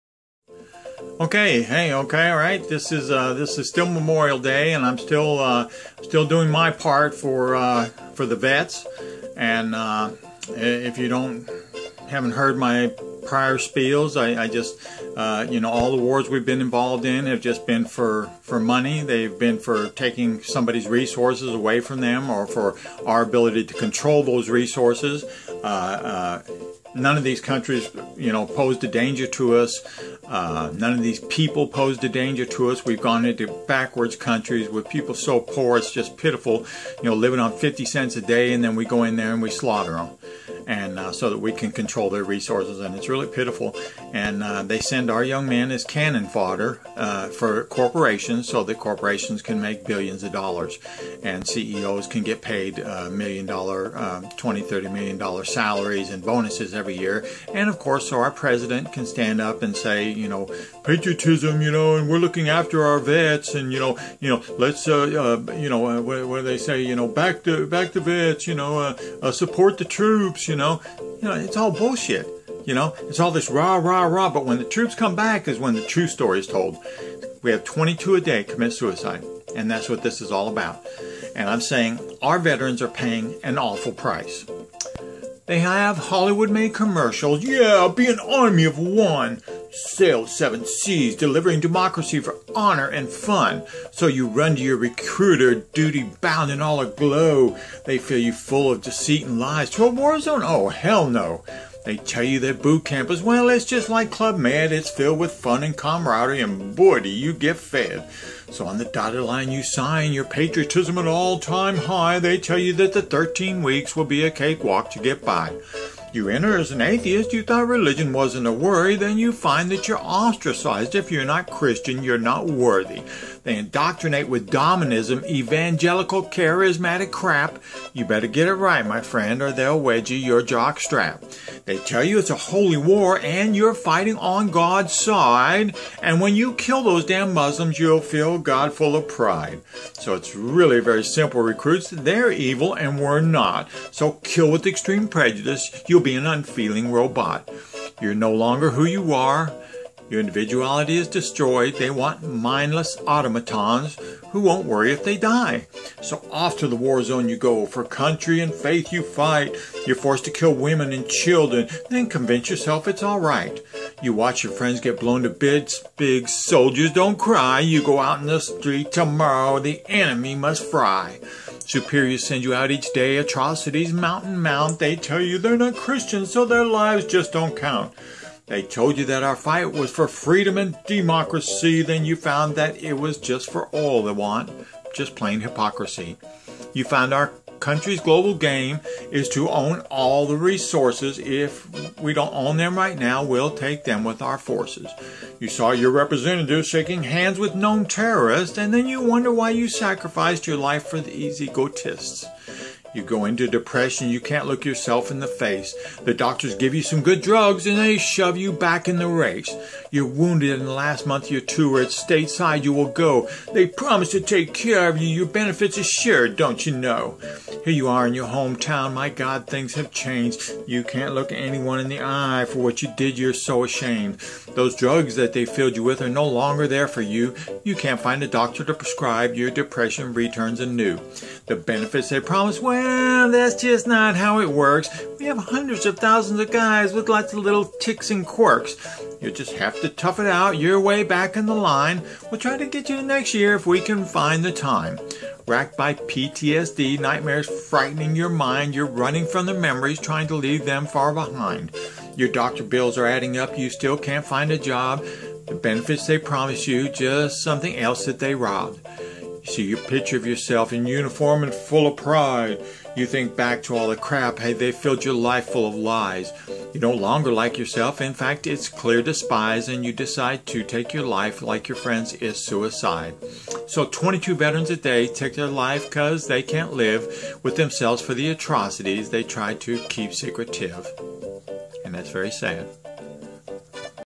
OUR VETERANS ARE PAYING AWFUL PRICE♡
Veterans used to promote a phony democracy with corporate CEO'S getting fat selling death and destruction.